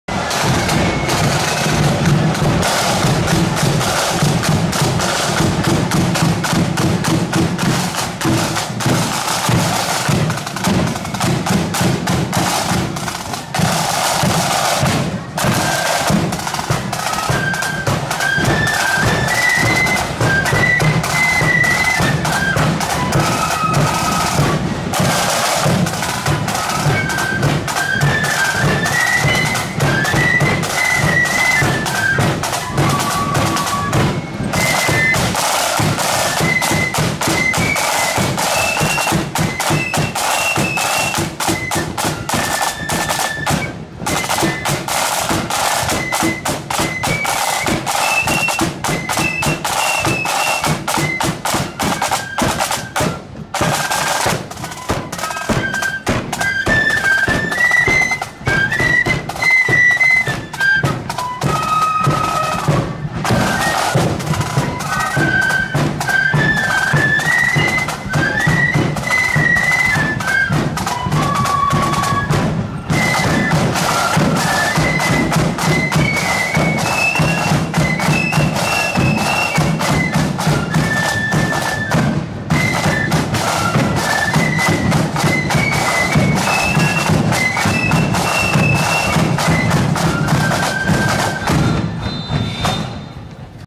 Giants Causeway Protestant Boys @ Pride Of The Bann's Parade ~ Coleraine ~ 26/05/23 (4K)
Giants Causeway Protestant Boys Flute Band parading through Coleraine at Pride Of The Bann Flute Band's annual parade which was held in the Co. Londonderry town on Friday 26/05/2023.